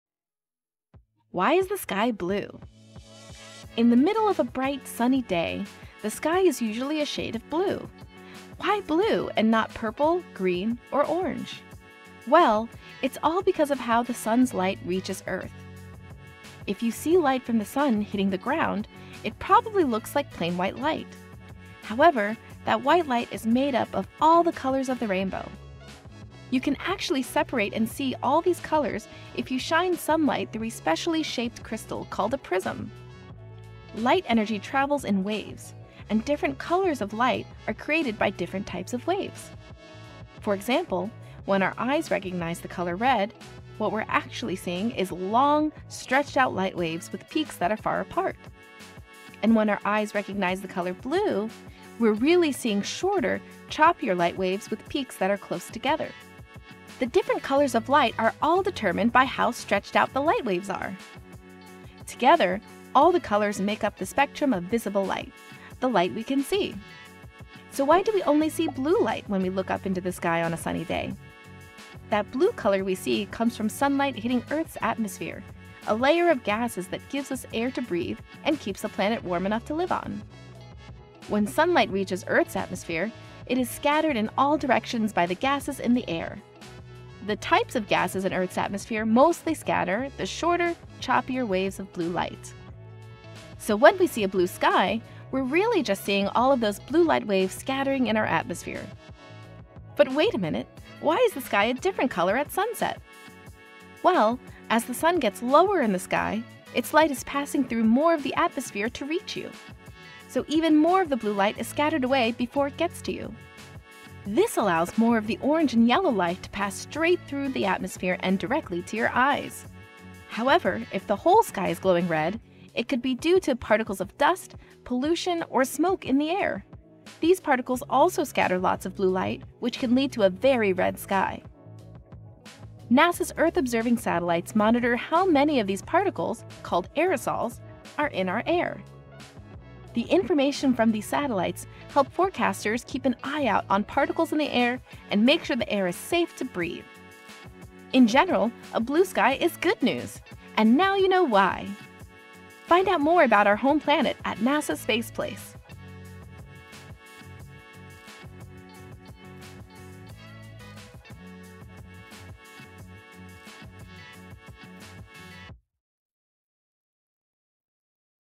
Why Does the Sky Appear Blue?
For what reason is the sky blue and not purple, green or orange? It's all a result of how the Daylight arrives at Earth!
Discover the captivating science that makes our sky appear blue! Uncover the secrets of atmospheric scattering and the wavelengths of light responsible for the stunning blue hue we see above. Dive into the world of physics and meteorology as we explore why the sky appears blue during the day. Join us on this illuminating journey to unravel the mysteries of our beautiful blue sky.

Credits: NASA Space Place